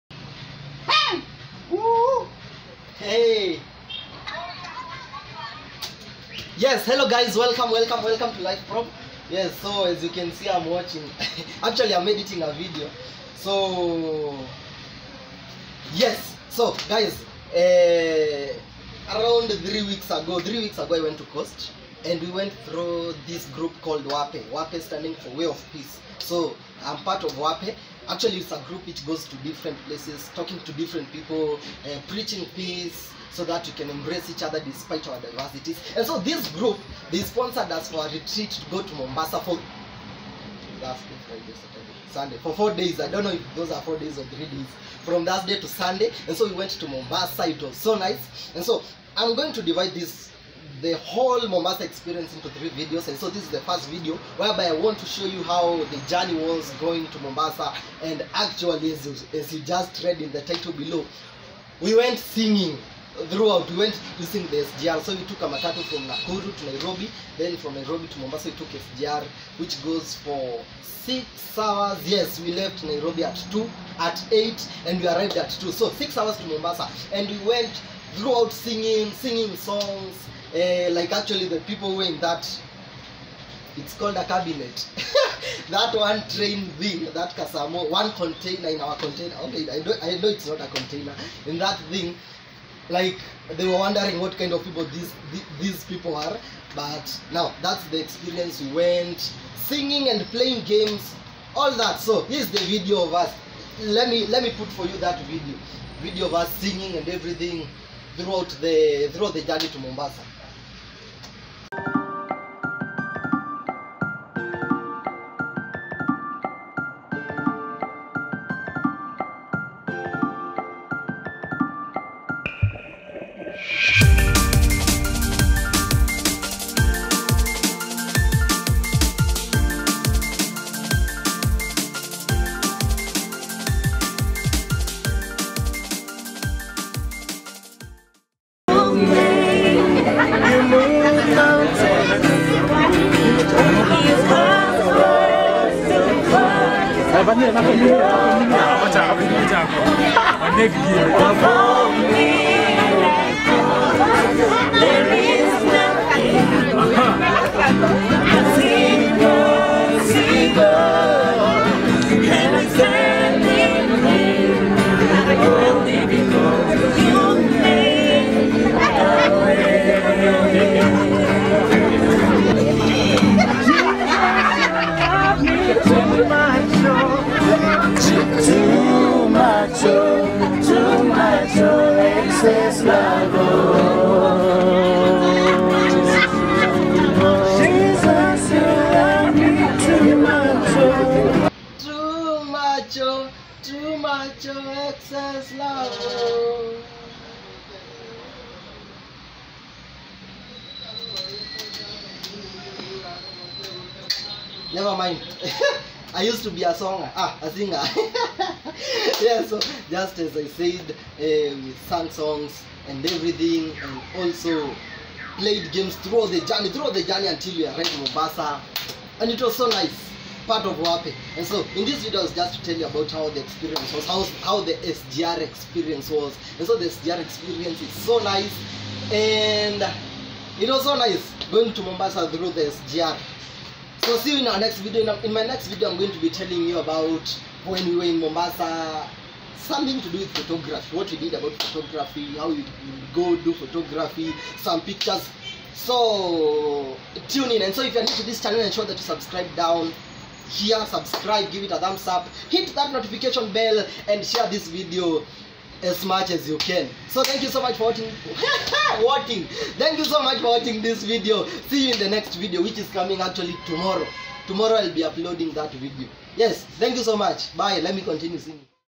FIRST TIME IN THE SGR ON OUR WAY TO MOMBASA
Heeeeeellllllllloooooo how are you doing. So this time (manuh) went to coast through a group called WAPE that stands for Way of Peace. As noisy as we naturally are we went singing cracking jokes playings games in the SGR till we arrived for Mombasa. 6 good hours to Mombasa.